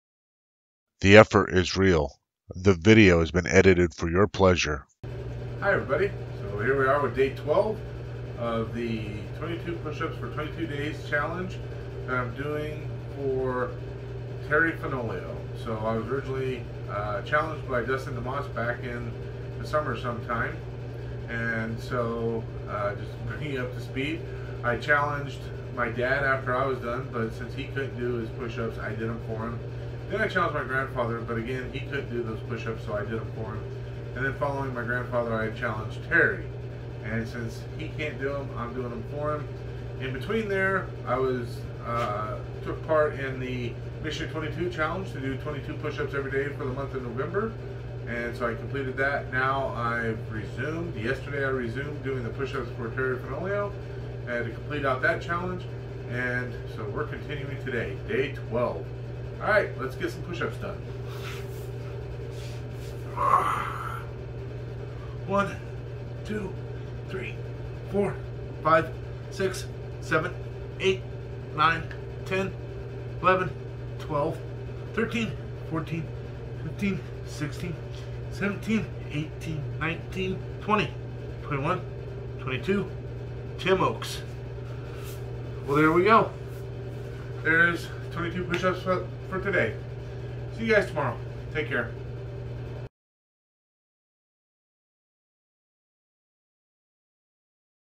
Day 12 of Terry's 22 Pushup Challenge
"Day 12 of the 22 day Pushup Challenge -  Veterans Suicide Prevention"
I am nominating Terry Cindy Fenoglio, but I am doing them instead.

I was challenged by Dustin DeMoss

For this challenge I'd like to nominate: Everyone watching
The rules are simple:
* Once you are nominated, your 22 days start the following day.
* Everyday record yourself doing 22 push-ups. Do them anyway you can. Effort is what counts...not form.
* Everyday you must nominate a different person.
* And finally have fun with this. This is just a simple interactive way to get the word out about a matter that we must continue to shed light on.
Studies show we lose 22 Veterans a day to suicide and the hope is this small act shows our teammates that they are never alone! We must continue to address the stigma surrounding mental health and proactively remove any guilt, shame, or resistance which stands in the way to seeking help.
Confidential Military Crisis Line: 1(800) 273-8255 (option 1) or Text 838255
Military One Source:
Suicide Awareness & Crisis Resources - 24/7 Support for the Military Community | Military OneSource
U.S. Department of Veterans Affairs:
If you would like to make a donation or you are a veteran looking for another place to reach out.

*These brave men and women raised their hands, swore an oath, put on uniform, and put their lives on the line to protect our freedoms and American way of life. We must show them we care, support them and that there's a better way. Stay in the fight! You are not alone! Your fellow brothers and sisters do have your back!
After all, our LOVE is the biggest weapon we have against this! God Bless you all and God Bless America!